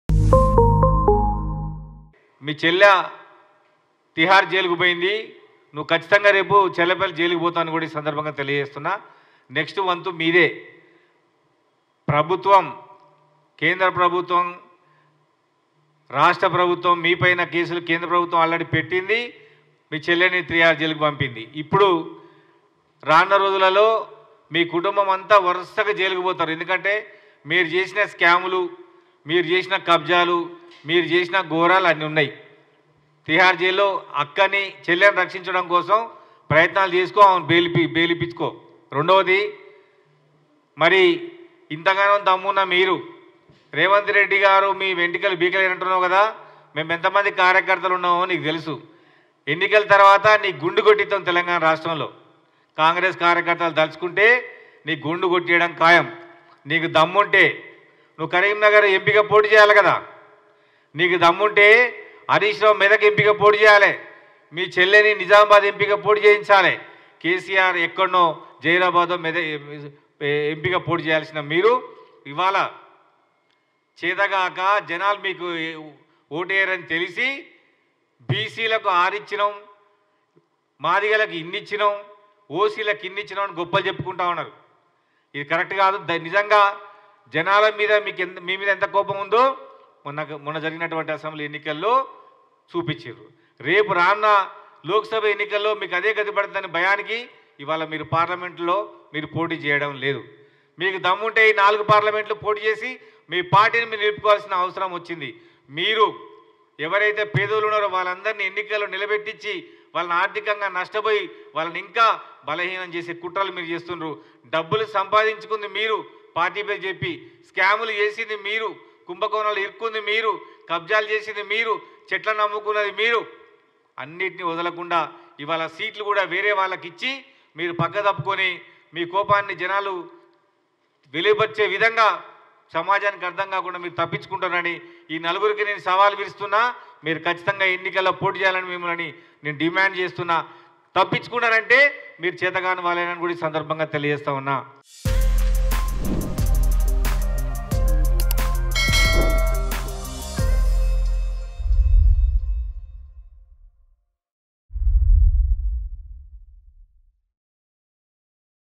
Congress Leader Pidamarthi Ravi fires On KTR: వెంట్రుక పీకుడు కాదు KTRకు గుండు కొట్టిస్తాం | BIG TV
Congress Leader Pidamarthi Ravi fires KTR: వెంట్రుక పీకుడు కాదు  KTRకు గుండు కొట్టిస్తాం | BIG TV

Congress leader Pidamarthi Ravi condemned KTR's comments on CM Revanth Reddy. Pidamarthi Ravi warned that KTR will be shot. Pidamarthi Ravi said that if you talk like you like Congress leaders, you will be punished.